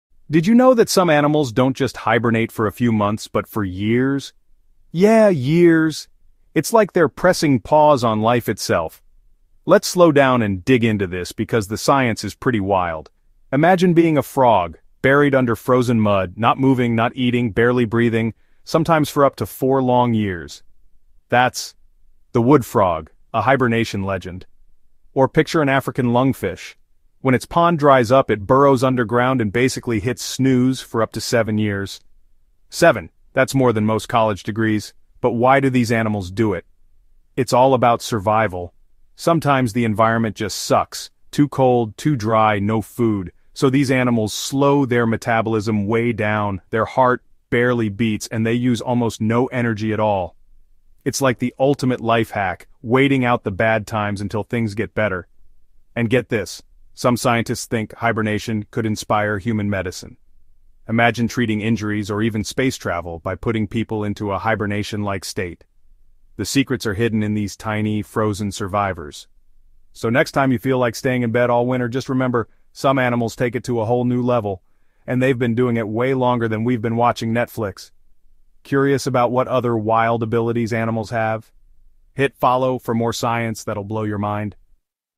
Why Some Animals Hibernate for Years, Not Just Months!! 😱
Discover the astonishing secret behind why some animals hibernate for years instead of just months! In this eye-opening video, we dive deep into the science of prolonged hibernation—how certain species survive extreme conditions by slowing their metabolism to a near standstill. Learn about the unique adaptations that allow these creatures to conserve energy and endure harsh environments for astonishingly long periods. Whether it’s bears, ground squirrels, or deep-sea creatures, this slow reveal uncovers nature’s incredible survival strategy. Don’t miss this fascinating journey into the world of extended hibernation!